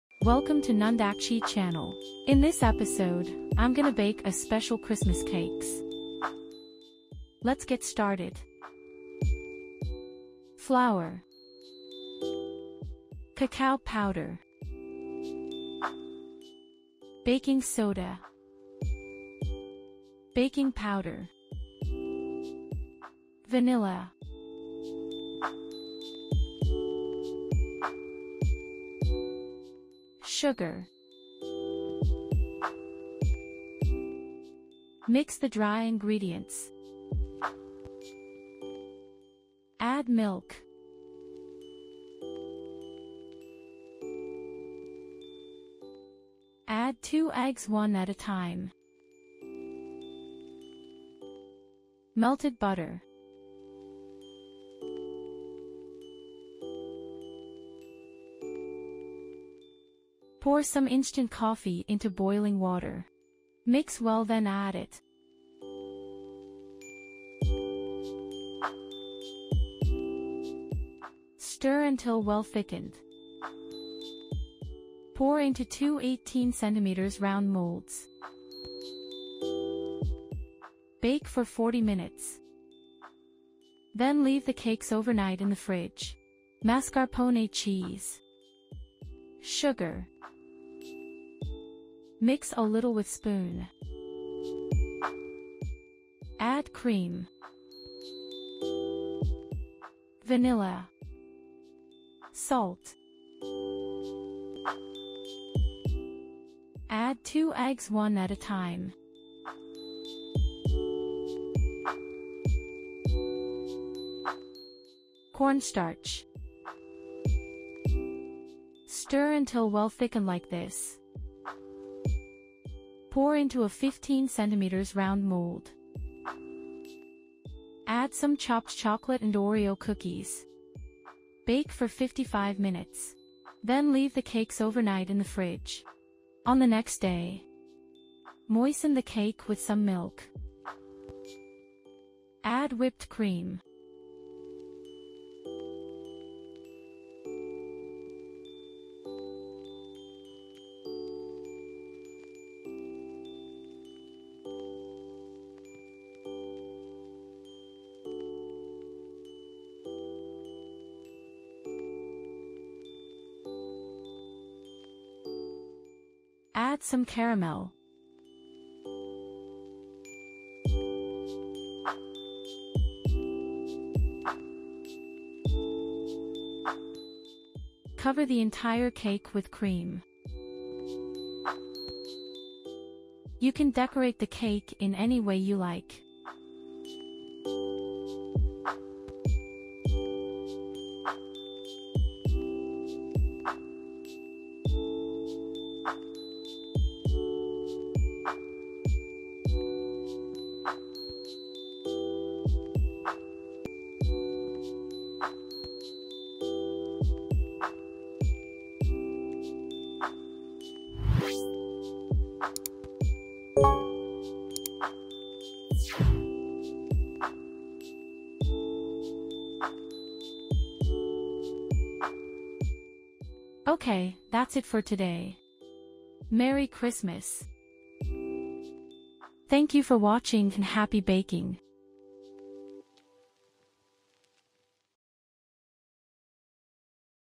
How to Make a Creamy Chocolate Oreo Cheesecake for Christmas🎄
▶️ What’s today's recipe about?
In this video, I'll show you how to make a delicious cream cake with chocolate and Oreo cheesecake, and decorate it semi-professionally for Christmas. This is my own recipe that I have perfected over the days, and I'm sure you'll love it. Although, It's not quite easy to make if you're an amateur, but it's rich in flavor, and perfect for the festive season.
Stay with me until the end of the video to guide this Christmas baking journey.
Good luck and Happy Holidays! 😊🎉🎄
Let's bake together 💪

▶️ Difficulty Level:
★★★  3/5

▶️Ingredients:
Ingredients for the cake (2 pieces) :
- 200 grams of flour
- 65 grams of cacao powder
- 1/4 tsp of baking soda
- 1 tsp of baking powder
- 1 tsp of vanilla
- 200 grams of sugar
- 150ml of milk
- 2 eggs
- 90 grams of melted butter
- 130 ml of boiled water
- 10 grams of instance coffee
Ingredients for the cheesecake:
- 400 grams of mascarpone
- 130 grams of sugar
- 80 grams of cream
- 1 tsp of vanilla
- 1/2 tsp of salt
- 2 eggs
- 10 grams of cornstarch
Ingredients for the filing:
- Some chopped chocolate or OREO cookies
- Caramel (200 grams of sugar + 100 grams of cream + 50 grams of butter)
Ingredients for Garnishment:
- Whipped cream (450 grams of whipped cream + 350 grams of cream cheese + 60 grams of powdered sugar)
- A little red fondant for the bow (optional)
- Custom edible color (optional)
- Fine and coarse white truffle (optional)

▶️ Instructions:
For Cakes:
- First, Mix the dry ingredients
- Add milk and 2 eggs one at a time
- Then pour the melted butter
- Pour some instant coffee into boiling water, mix well then add it.
- Stir until well thickened
- Pour into two separate 18 cm round molds
- Bake cakes for 35 /40 minutes at 165 °C / 329°F
- Then keep the cakes overnight in the fridge
For cheesecake:
- Add sugar to mascarpone and mix a little with spoon
- Add cream, vanilla, salt, 2 eggs one at a time, cornstarch
- Stir until well thickened like this
- Pour into a 15 cm round mold
- Add some chopped chocolate & OREO cookies
- Bake it for 55 minutes at 125 °C / 257°F
- Then leave the cheesecake overnight in the fridge
For continue on the next day:
- Moisten the cakes with some milk
- Start to cover the entire of cakes with whipped cream
- Make the caramel and pour it in the middle of the cake.
- You can decorate the cake in any way you like
Enjoy your freshly baked and enjoy Holliday Season!

▶️ Tips & Tricks:
- Preheat the oven for 15 minutes before baking
- Pay attention to the times mentioned for resting the dough in the instructions
- Do not use an electric mixer in any of the steps.
- While covering with cream, if the room temperature was not cold, put the cake in the refrigerator from time to time and continue again after an hour so that the cream doesn't lose its shape.
- After finishing, rest the cake overnight in the fridge and then cut for eat

♥️ Unleash your inner baker and enjoy it ♥️
I hope you enjoy this video and give this recipe a try.
I’m excited to receive your comments or questions.

▶️▶️▶️Chapters on this Video ▶️▶️▶️
0:00 Preview
0:11 Cake Preparation
1:26 Cheesecake Preparation
2:16 Stacking the layers
2:51 Covering & Decorating
3:43 Final Result